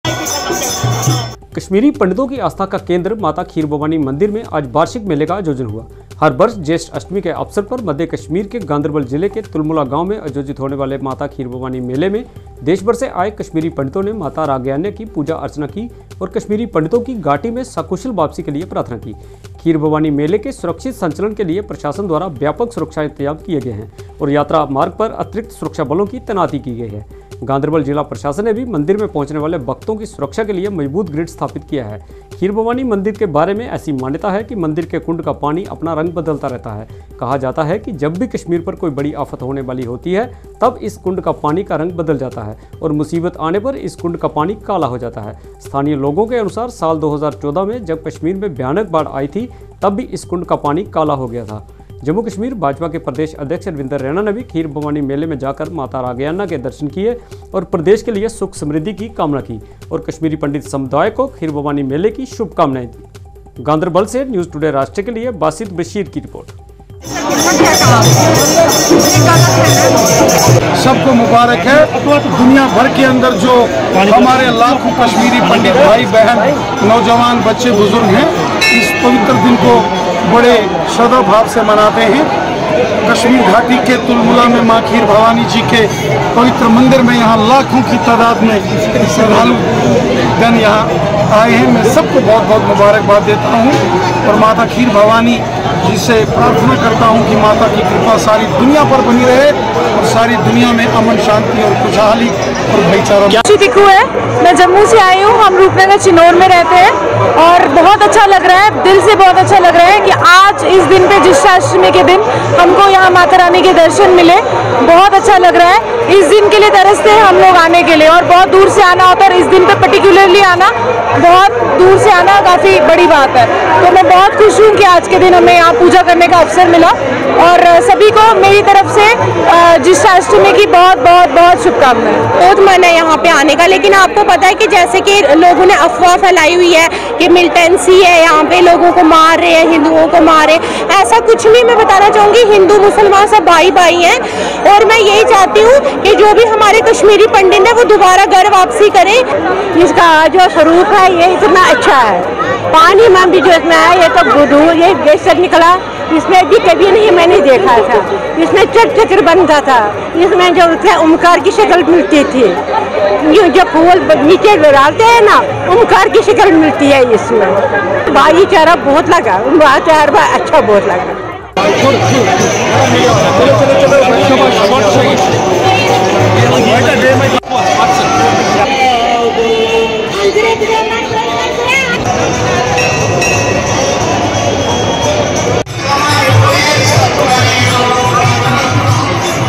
Over 30,000 devotees paid obeisance at Mela Kheer Bhawani Tulmulla, Ganderbal
मेला खीर भवानी में 30,000 से अधिक श्रद्धालुओं ने की माता राग्याना की आराधना, हर वर्ष ज्येष्ठ अष्टमी पर गांदरबल के तुलमुला गाव मे आयोजित होता है खीर भवानी मेला